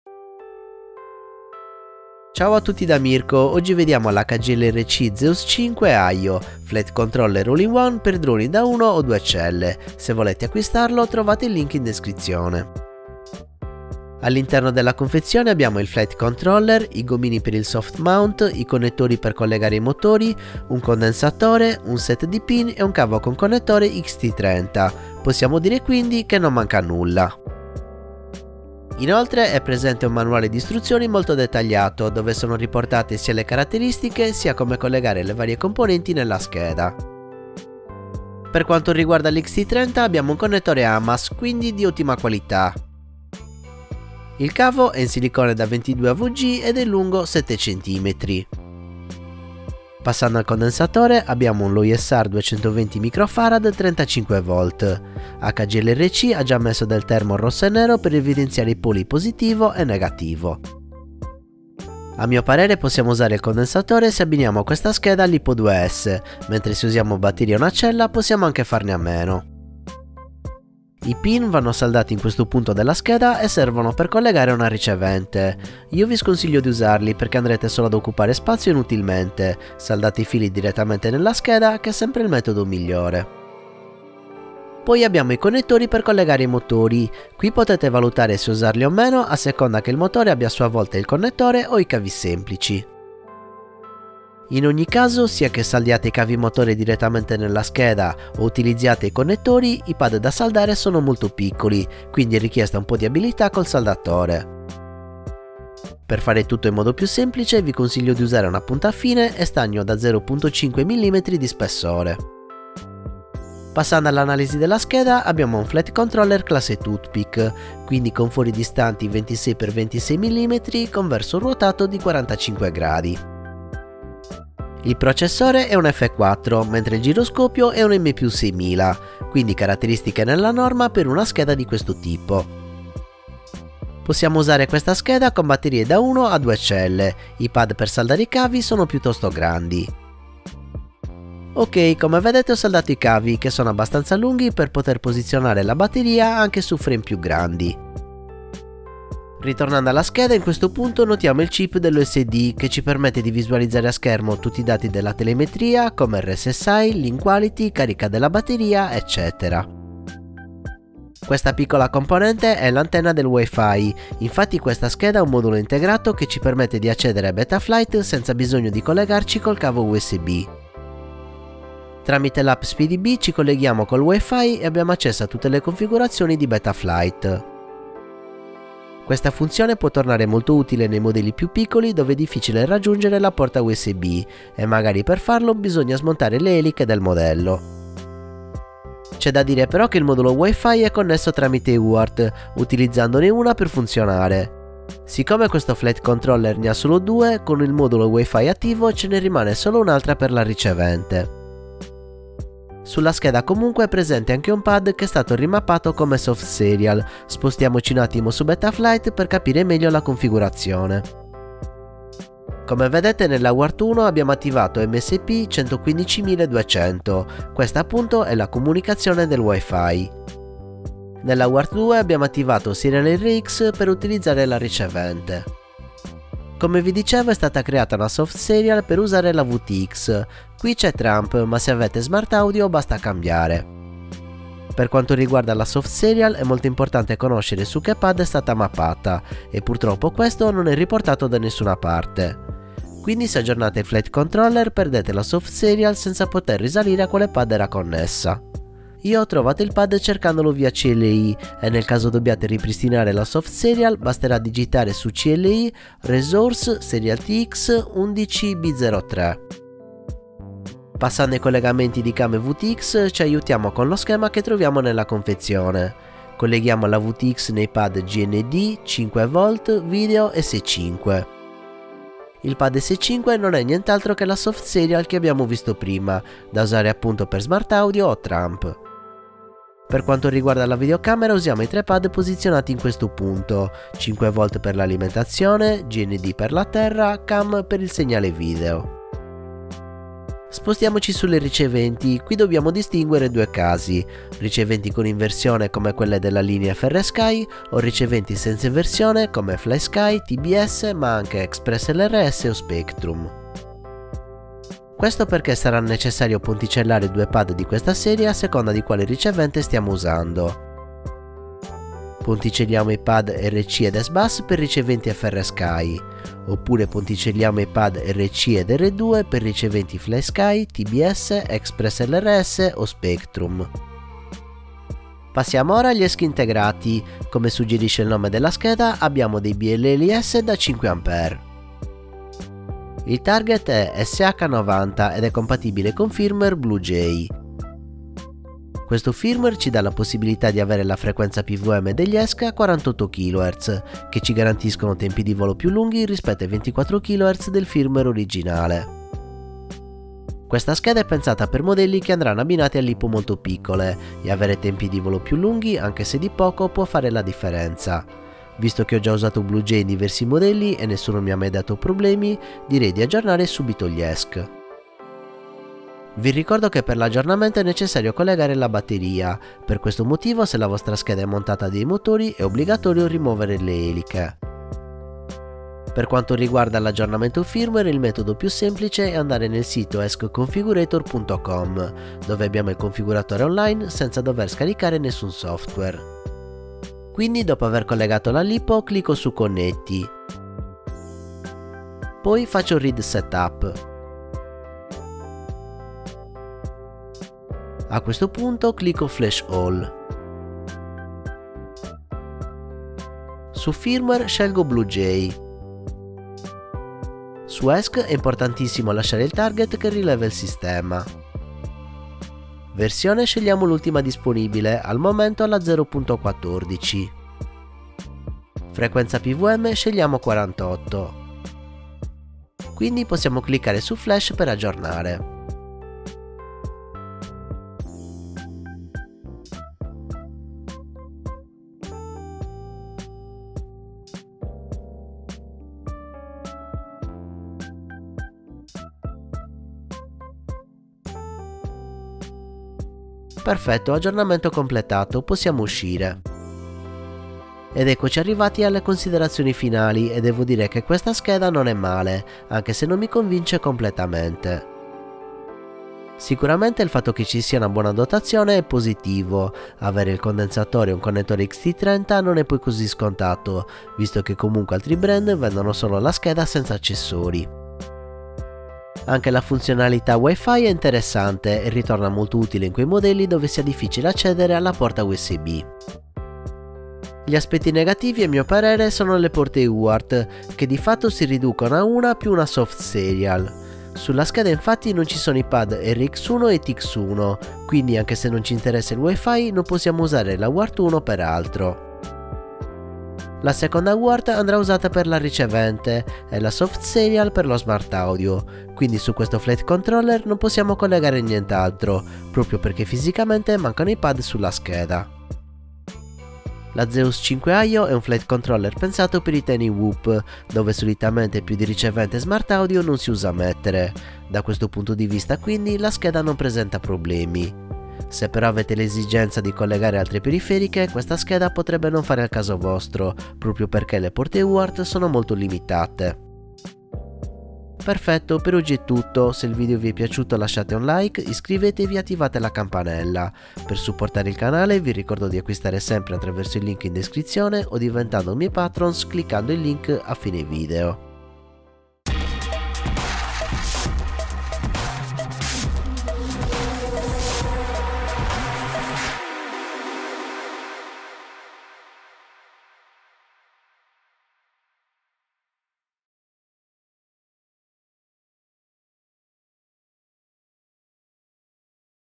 HGLRC Zeus5 AIO - Flight Controller Review
▶ Timeline
00:00 Intro & Unboxing
01:53 Specifiche FC
02:40 WI-FI
03:22 UART e SOFTSERIAL
04:20 Collegare CAM e VTX
04:49 Collegare le riceventi
05:24 ESC 5A
06:15 Aggiornamento ESC con BlueJay
07:27 Conclusioni